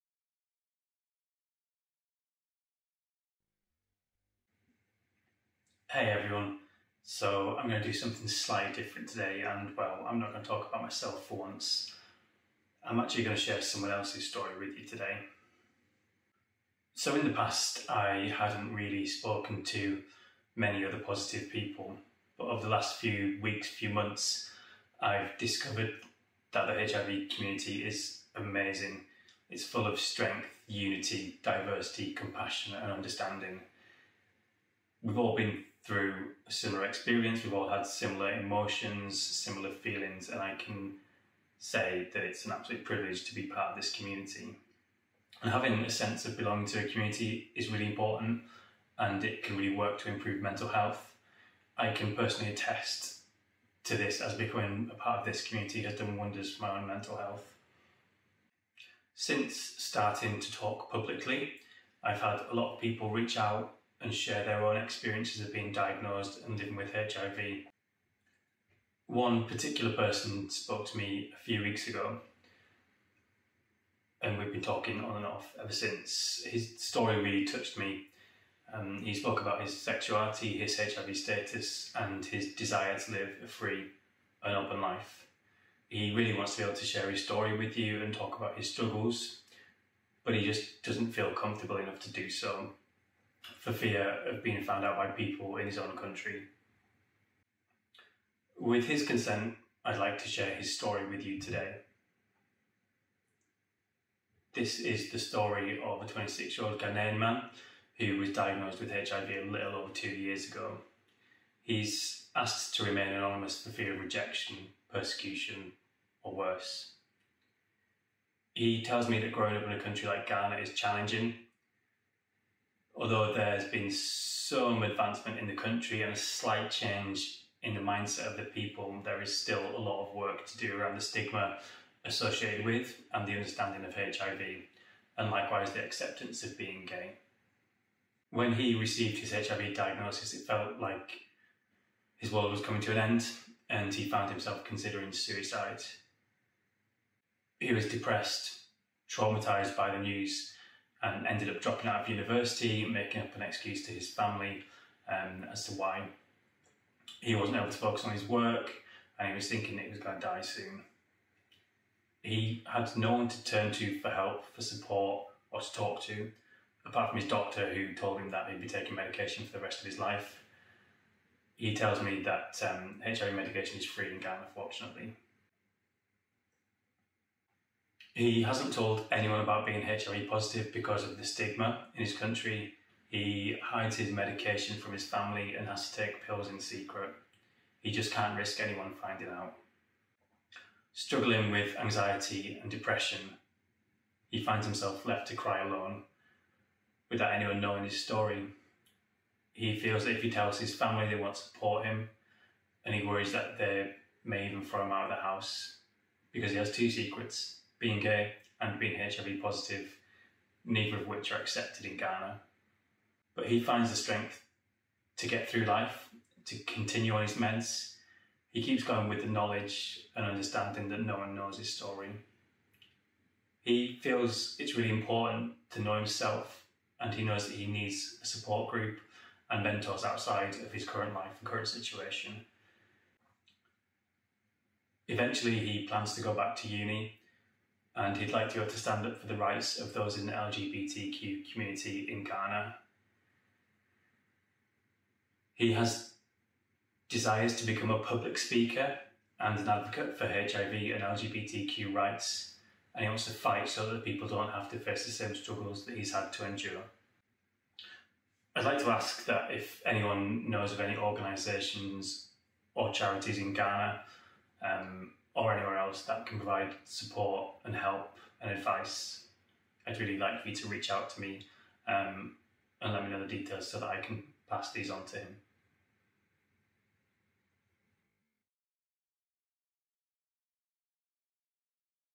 E's HIV Story
It’s great being part of a community. The HIV community in particular has been amazing to me.

A lot of people have reached out to me recently to share their own story, their own experiences of being diagnosed and living with HIV.

Today, I’m sharing someone else’s story with you. This is the story of “E”, who asked me to share his experience and to remain anonymous. These are his words.